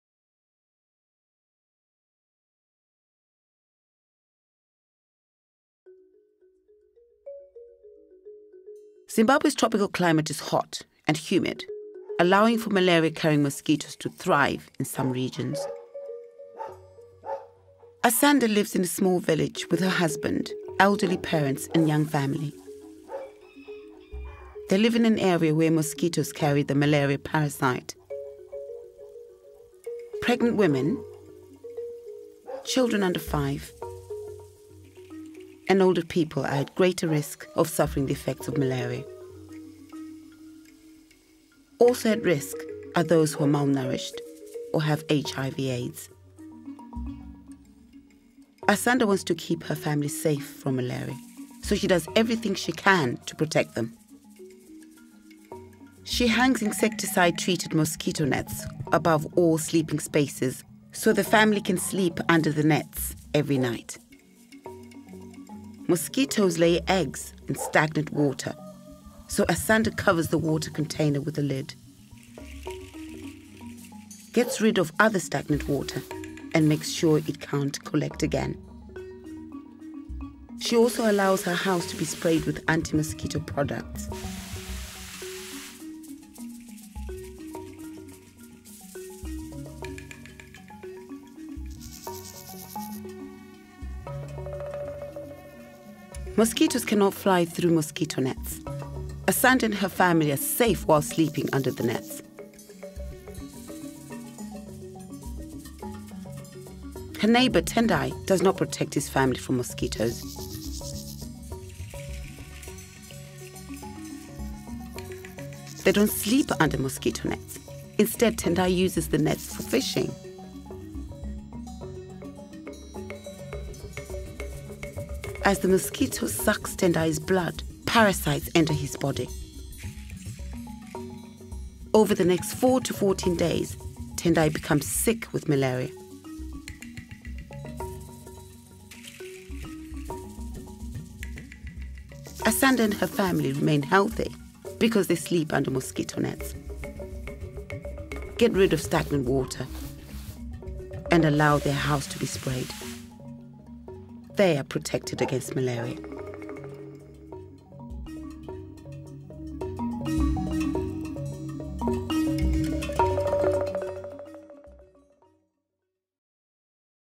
2 Medical Aid Films' How to Prevent Malaria - Bond Corporate Partnerships Award submission
Medical Aid Films has partnered with Econet Wireless, Zimbabwe's biggest mobile operator, to reach health workers and communities through their mobile phones.

They have created mobile-friendly animations focusing on the prevention, diagnosis and treatment of malaria, available in English, Shona and Ndebele. The films are available to watch for free on the zero-rated Econet Health website, with a link disseminated via blast SMS to all subscribers.

This submission is part of the Bond International Development Awards. The winners will be announced on 26 February 2018. See the rest of the Corporate Partnerships Award submissions